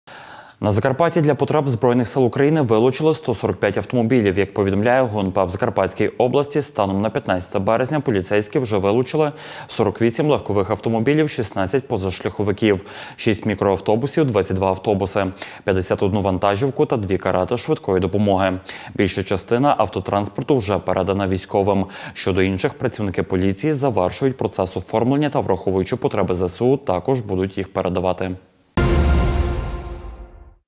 Потреби для армії. Вилучають автомобілі
На Закарпатті для потреб ЗСУ вилучили 145 автомобілів.Як повідомляє ГУ НП в Закарпатській області, станом на 15 березня поліцейські вже вилучили: 48 легкових автомобілів, 16 позашляховиків, 6 мікроавтобусів, 22 автобуси, 51 вантажівку та дві карети швидкої допомоги.Більша частина автотранспорту вже передана військовим. Щодо інших – працівники поліції завершують процес оформлення та, враховуючи потреби ЗСУ, також будуть їх передавати.